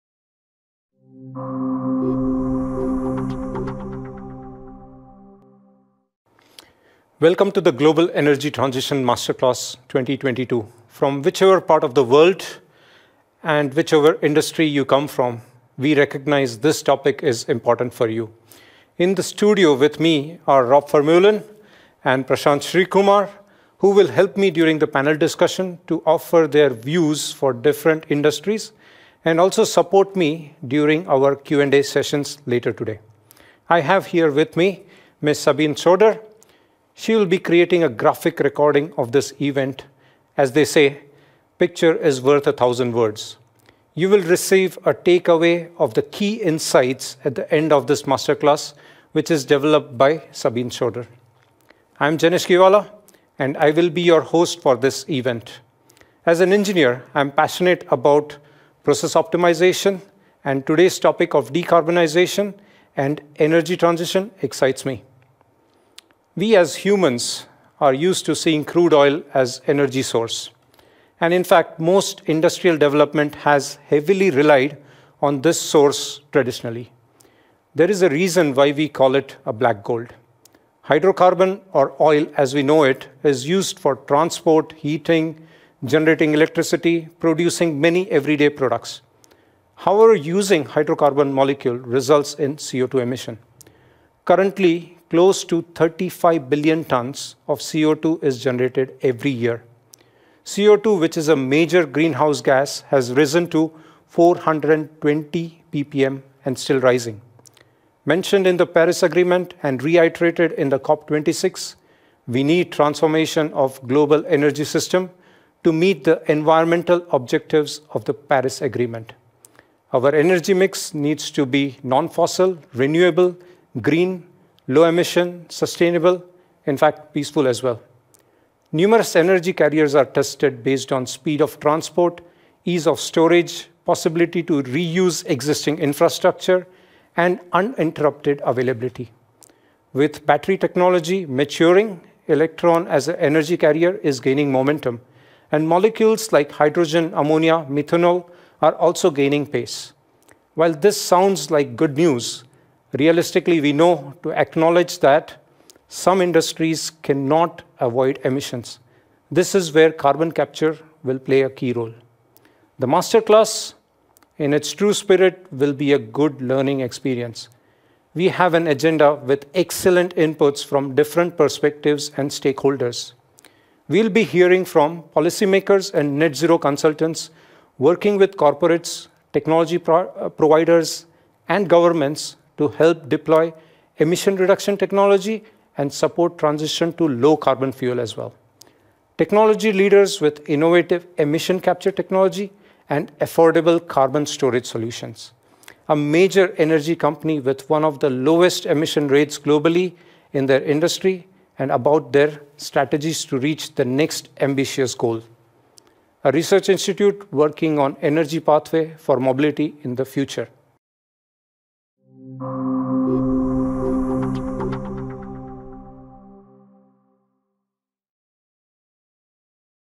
Energy Transition | Introduction to Global Energy Transition Masterclass
Want to get new ideas on how to reach the Net-Zero goal? Watch the entire playlist and get practical insights from experts working on the Net-Zero goal and listen to cross-industry panel discussions and exchanges with industry peers.

Heavy industries such as oil & gas, cement, metal, and chemical production account for 40% of global energy demand. One quarter of all CO2 emissions come from these industries and are hard to abate. The demand for energy, cement, steel, and chemicals will further increase with continuous urbanization and growing world population. If we are going to reach global climate goals, we need to improve efficiency and significantly reduce CO2 emissions.

Numerous pilot projects worldwide answer to this call. Get insights and gain new knowledge and ideas from leading companies around the globe who have gained first hand experience in reducing CO2 impact on our environment.

This video is the introduction of a 12-part video series of our Endress+Hauser Global Energy Transition Masterclass, which took place in March 2022.